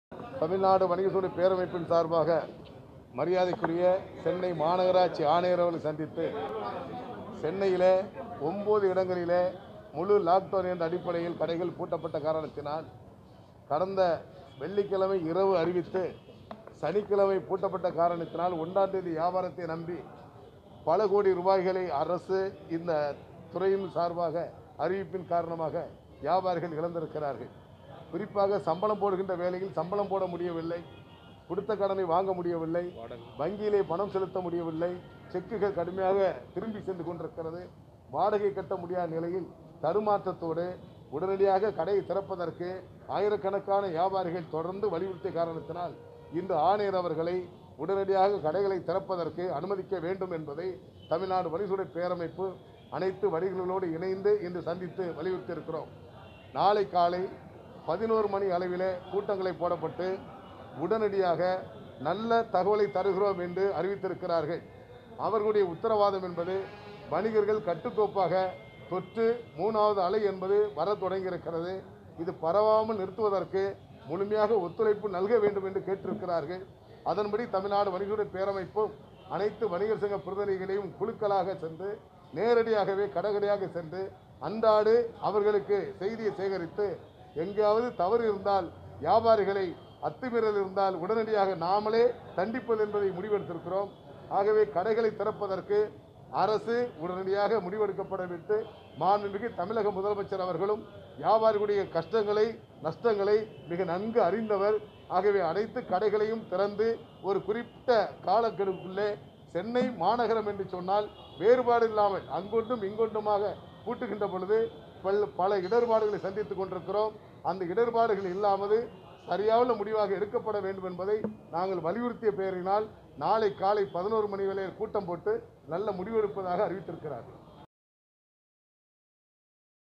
மூடப்பட்டு இருக்கும் கடைகளை உடனடியாக திறக்க வேண்டும் | Chamber Of Commerce | ETV Bharat Tamil Nadu
சென்னையில் மூடப்பட்டிருக்கும் கடைகளை உடனடியாக திறக்க வேண்டுமென தமிழ்நாடு வணிகர் சங்க பேரமைப்பு தலைவர் விக்கிரமராஜா வலியுறுத்தியுள்ளார்.